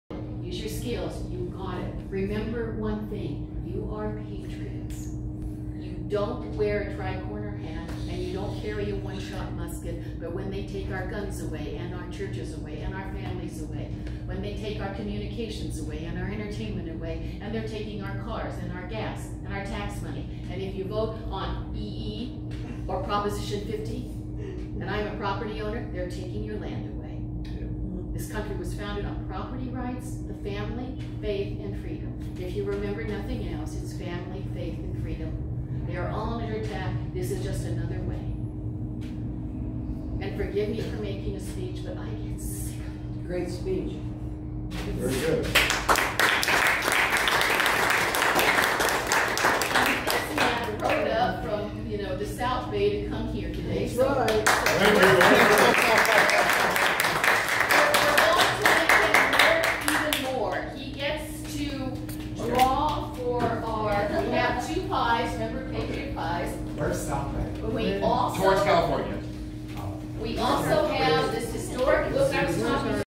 San Fernando Valley Patriots, Final Word
Karen Kenny, the host and leader for the San Fernando Valley patriots, gave an excellent final set of comments. I was so pleased and so honored that I can speak to this vocal and well active community of patriots who love this country  And the rule of law.